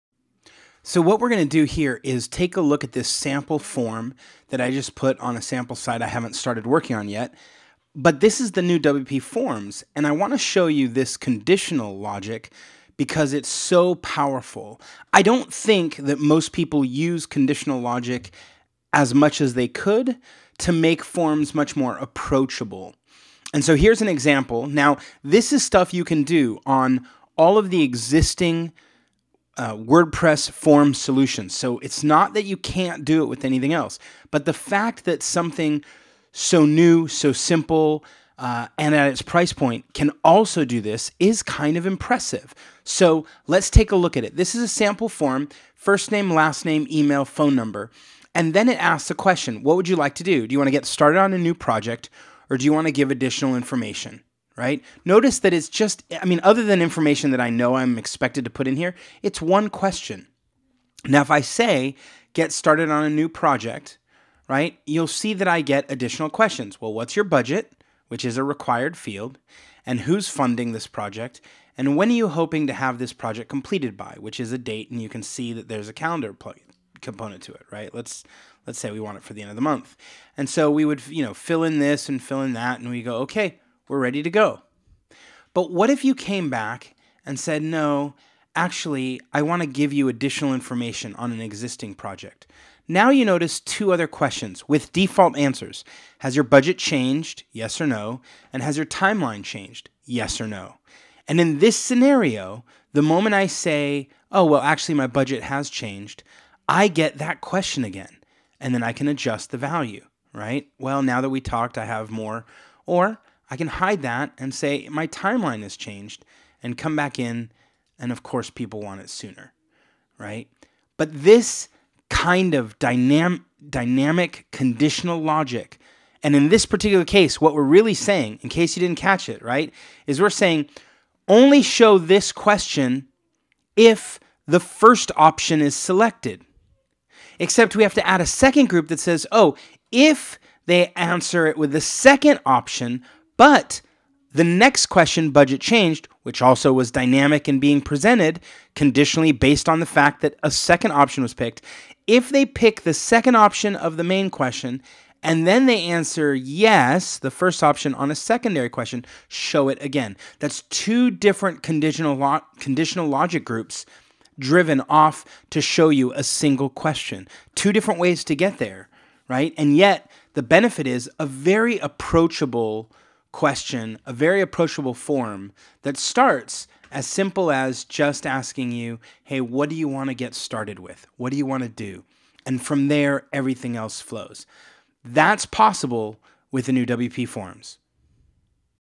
Conditional Logic with WPForms
There’s a new forms solution for WordPress and here’s how it handles conditional logic.